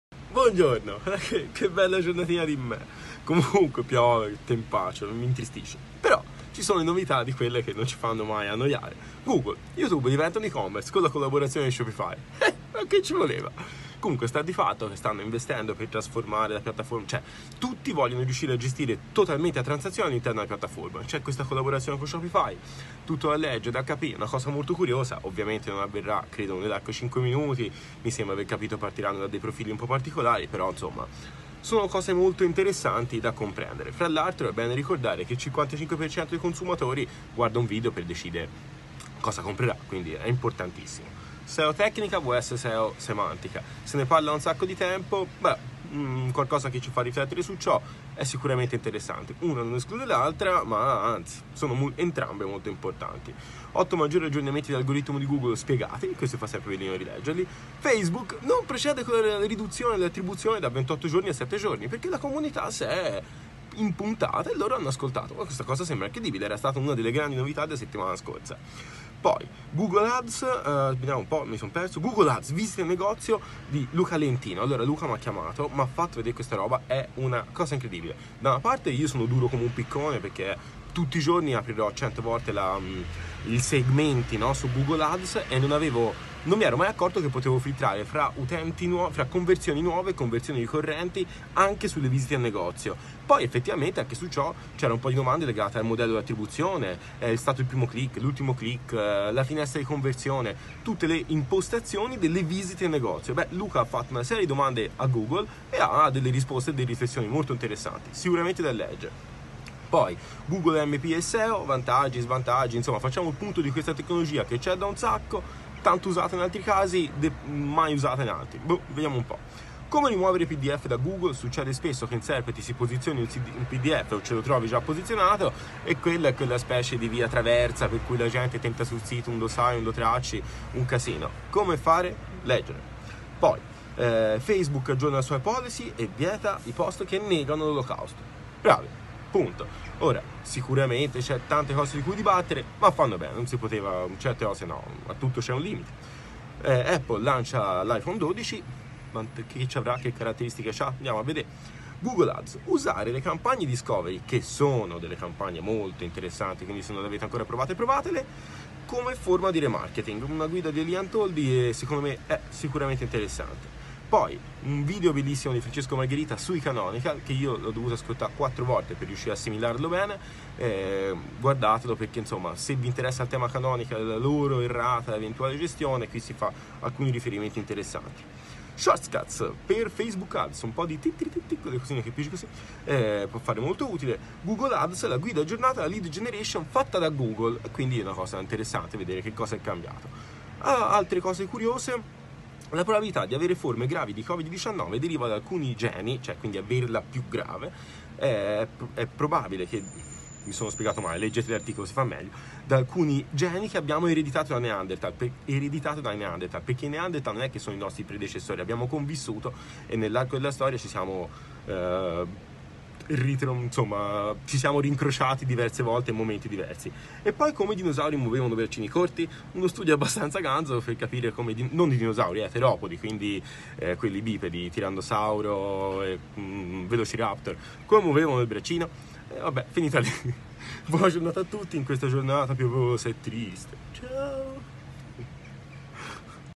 News Marketing del 14 Ottobre 2020 by Fabio Antichi
🍀 News: tornano le grandi novità 😛
E tante altre cose curiose 😅😅
✅ Google: Youtube diviene un ecommerce con l'aiuto di
🥜 55% consumatori usa i video per decidere
✅ Seo Tecnica vs Seo Semantica Marilena Colasuonno
✅ 8 maggiori aggiornamenti dell'algoritmo di Google spiegati
🥜 Facebook NON procede alla riduzione della finestra di
✅ Google ADS Visite in negozio e clienti ricorrenti Luca Lentino
✅ Google AMP e SEO: pro e contro
✅ Come rimuovere i PDF da Google Giovanni Sacheli
✅ Facebook aggiorna la sua policy e vieta i post che negano l’Olocausto Massimiliano Leva
✅ Apple lancia l’iPhone 12
✅ Google ADS Discovery come remarketing Elia Antoldi
✅ Canonical pensati bene Francesco Margherita
✅ Shortcuts for FacebookAds! 🙌 Bram Van der Hallen
✅ GoogleAds: Guida aggiornata alla Lead Generation Tommaso Zavattaro
🔹 La probabilità di avere forme gravi di Covid-19 deriva da alcuni geni ereditati dai
🔹 Come i dinosauri muovevano i braccini corti?